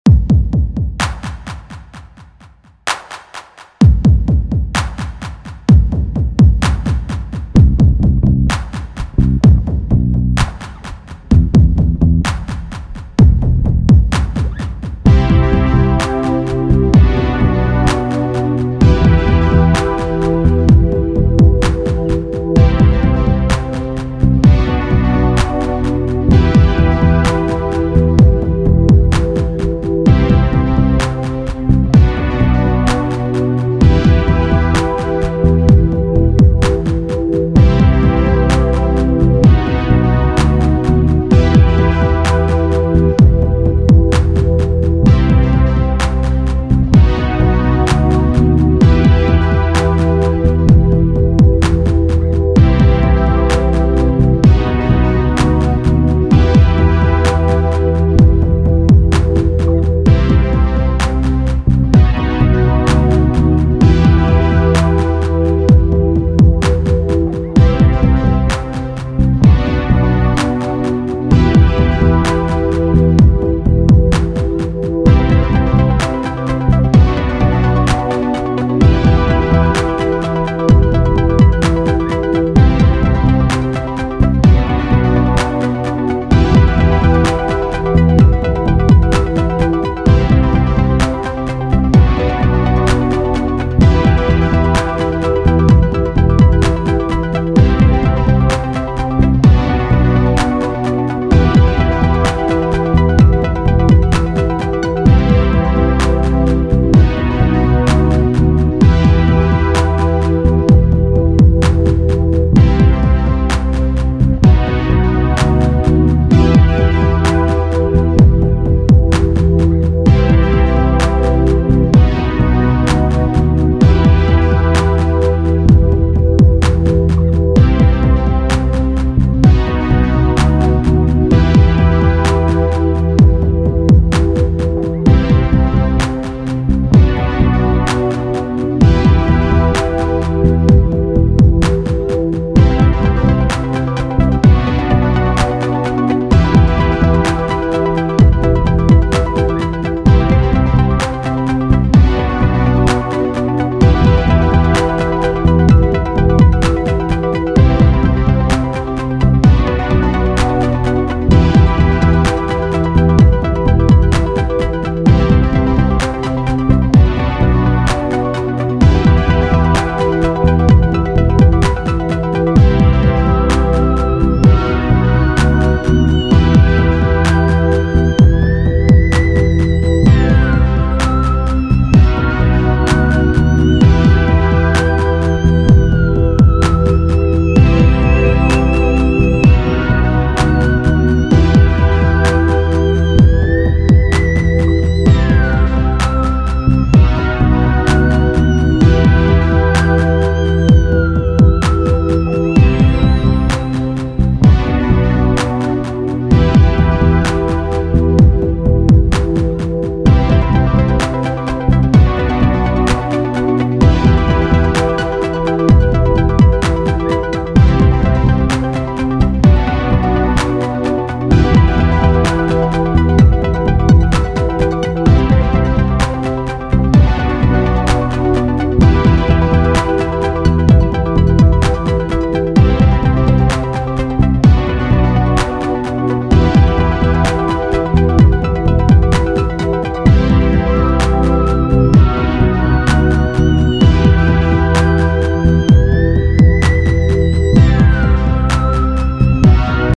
le tepidarium @ les thermes de Constantin à Arles
le tepidarium (bains tièdes) des thermes de Constantin (IVe) à Arles